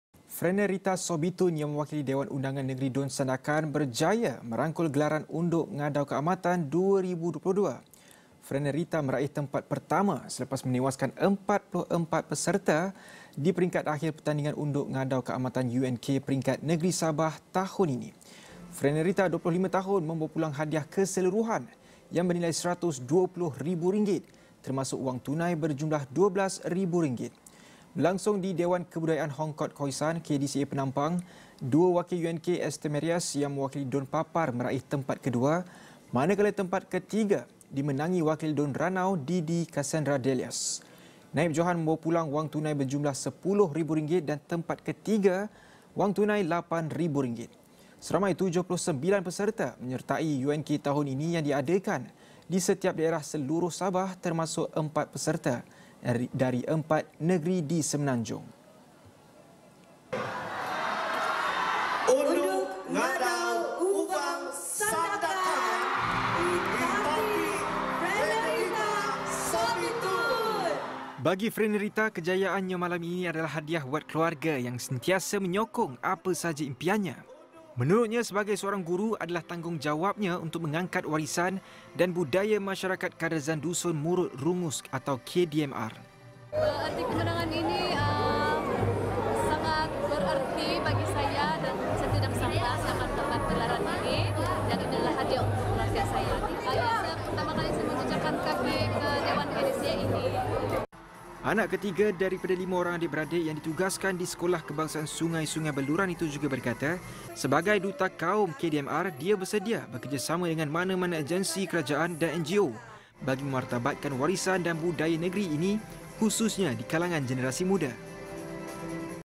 Unduk Ngadau | Frenerietta Sobitun dinobat juara UNK 2022
Frenerietta Sobitun yang mewakili Dewan Undangan Negeri (DUN) Sandakan berjaya merangkul gelaran Unduk Ngadau Kaamatan 2022.

Frenerietta meraih tempat pertama selepas menewaskan 44 peserta di peringkat akhir pertandingan Unduk Ngadau Kaamatan (UNK) peringkat negeri Sabah tahun ini.